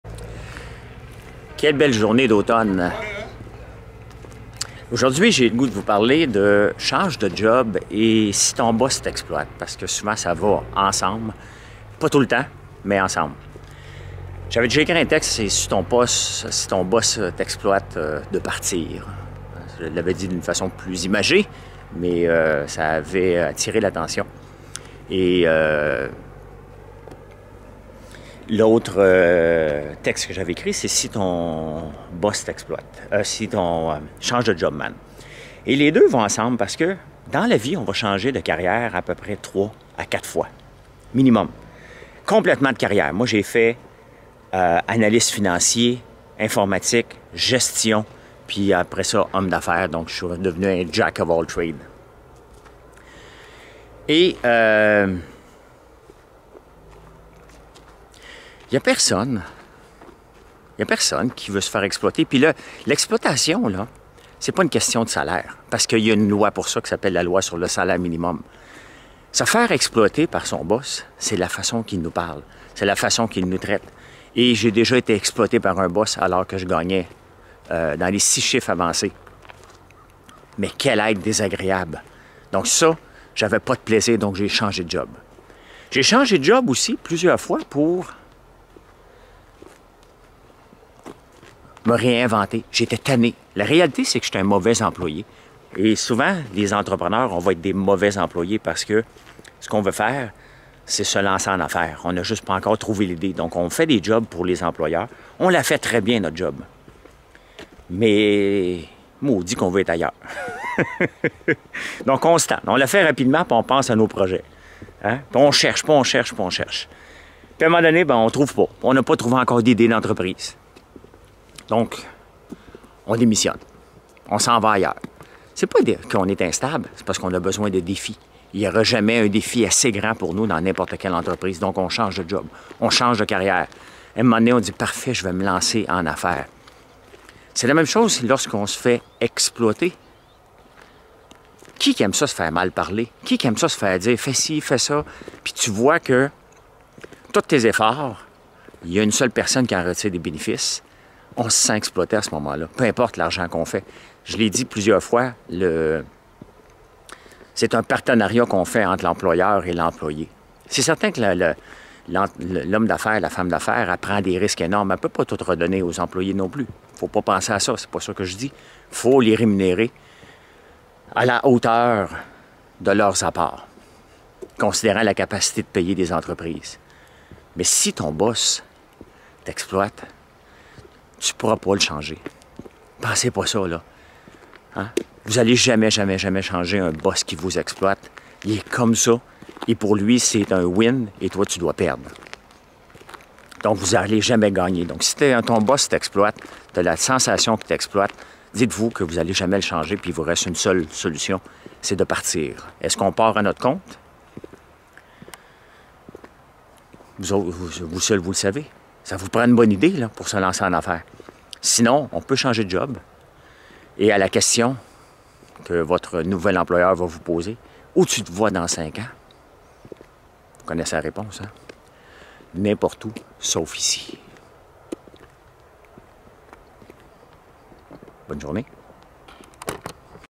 Votre patron est toxique? ou encore si ton boss t'exploite...
Que doit-on faire si notre patron est toxique? Je vais vous donner la réponse tout de suite... Ah puis non! allez écouter la vidéo à la place ;)

Pourquoi change-t-on de carrière ou d'emploi?
- Parce que nous sommes des mauvais employés?
- Parce que notre boss nous "exploite"
- Parce que on avance pas comme on voudrait?

Bon visionnement ;)

Courte Bio
François Lambert est un “serial entrepreneur” qui s’amuse chaque jour en faisant la promotion de ses friandises à l’érable!